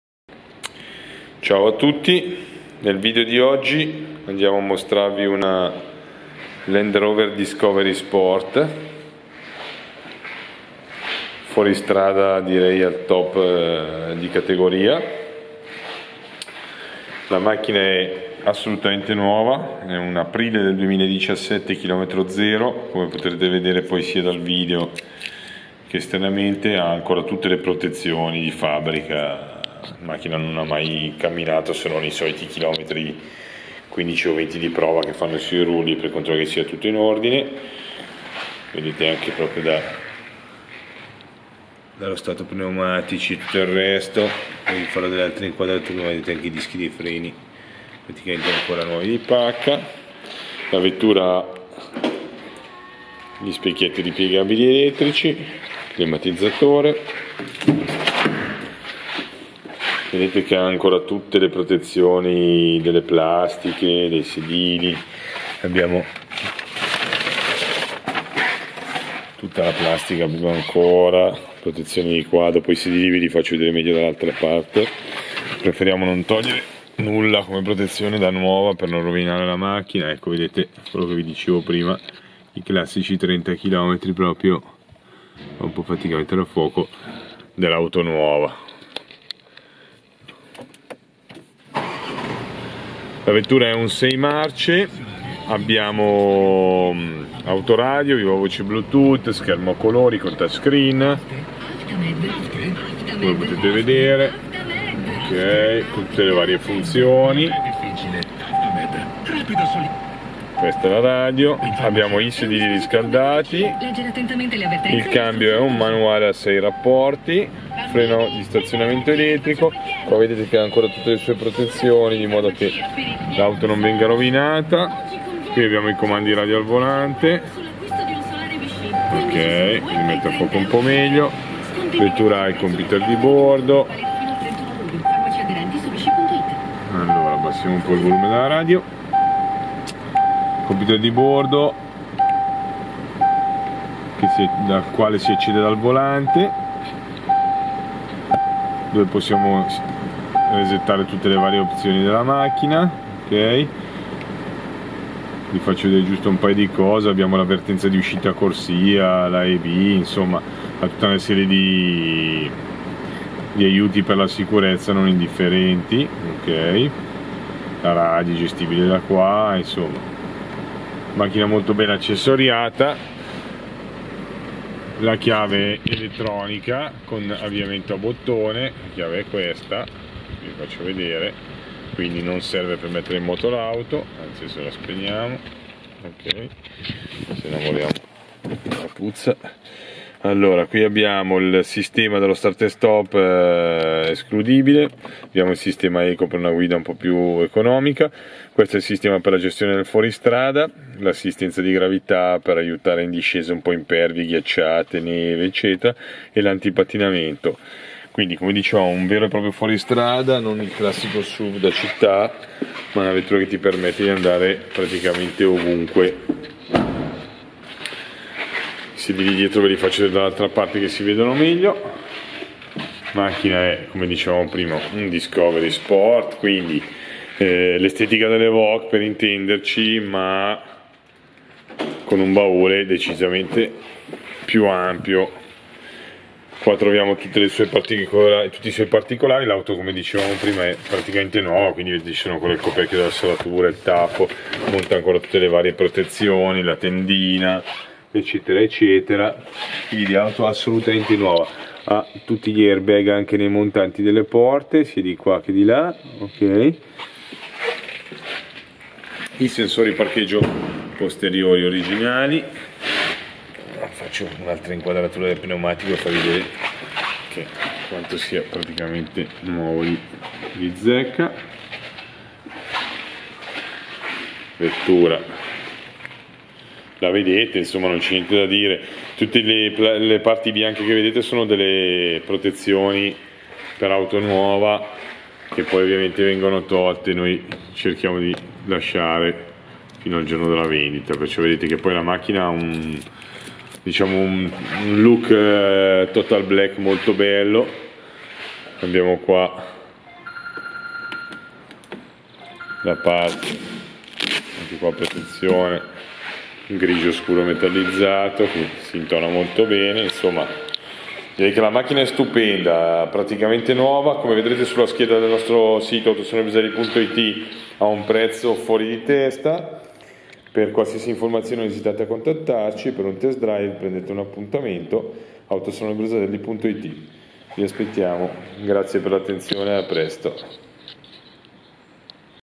LAND ROVER Discovery Sport Pure 2.0 TD4 150 CV 4WD - KM ZERO
Land Rover Discovery Sport Pure TD4 150CV 4WD, trazione integrale, non perderti questa meraviglia, km 0 immatricolata 4/2017, 2 anni in garanzia ufficiale Land Rover.

Prezzo di listino: 40.786 €

Optional extraserie:

cerchi in lega da 17” Design
specchietti retrovisori ripiegabili elettricamente
sedili anteriori riscaldati
tetto interno Cirrus
poggiatesta sedile posteriore centrale
volante multifunzione
Computer di bordo
tendina vano bagagli
sensori di parcheggio posteriori
pacchetto Climatizzazione
USB, Aux
vernice metallizzata